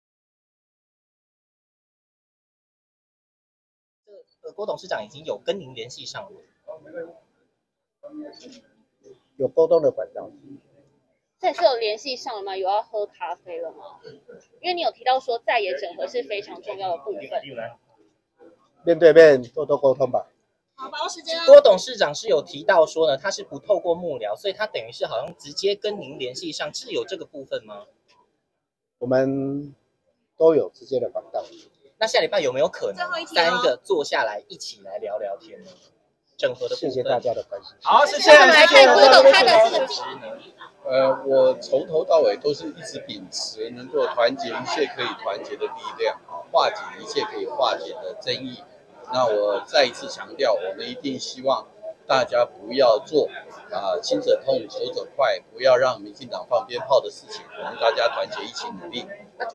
侯友宜出席國代挺侯後援會|中央社即時影音
國民黨總統參選人侯友宜25日在台北天成大飯店出席「侯友宜競選總統國民大會代表後援會成立大會」，會前媒體提問是否與鴻海集團創辦人郭台銘聯繫，侯友宜表示，有溝通的管道。（中央社記者裴禛攝）